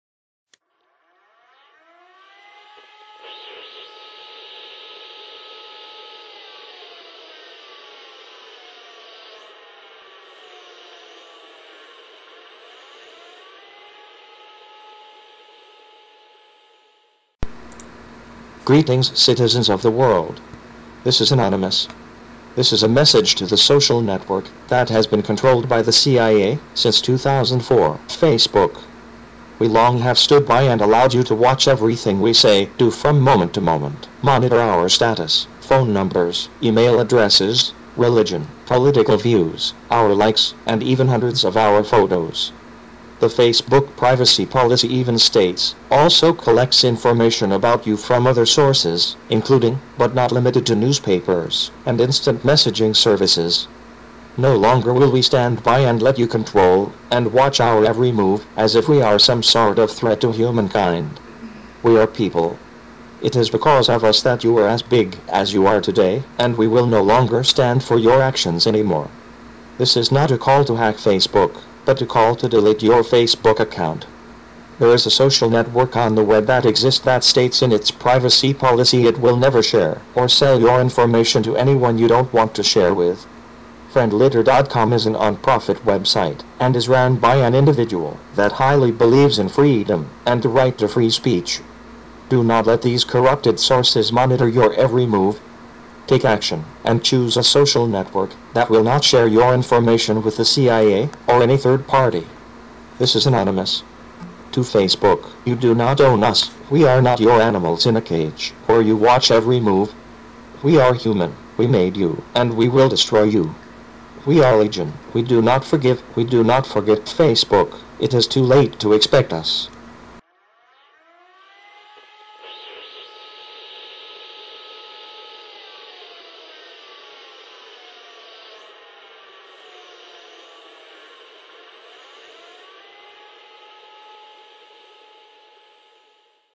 Anonymous message to facebook
Delete the monitoring social network facebook. If you still wish to communicate with others, use the social network that will not share or sell your information to anyone you do not wish to share with. friendlitter.com